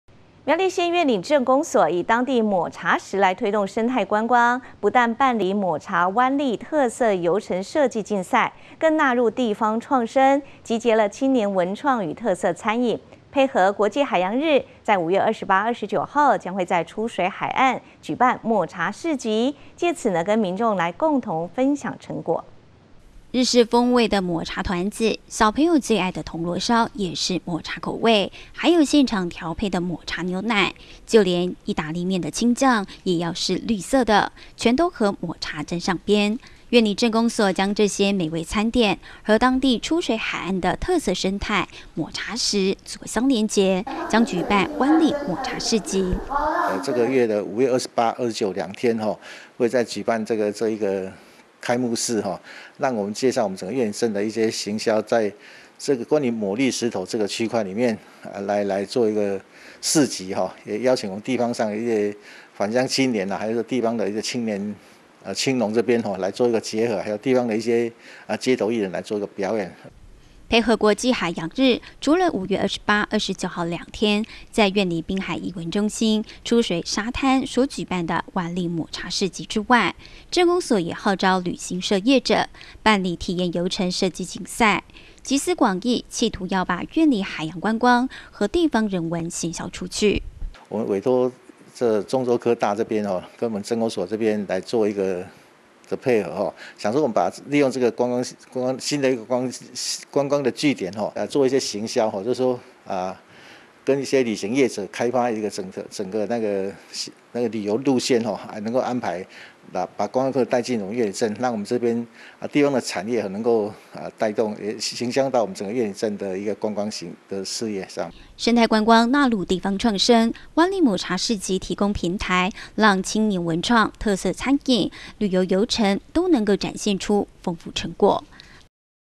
苑裡推動抹茶石生態觀光 市集展成果│T-NEWS聯播網
苗栗縣苑裡鎮公所以當地抹茶石推動生態觀光，不但辦理抹茶灣麗特色遊程設計競賽，更納入地方創生，結集青年文創與特色餐飲，配合國際海洋日，5/28、29將在出水海岸舉辦抹茶市集，藉此與民眾共同分享成果。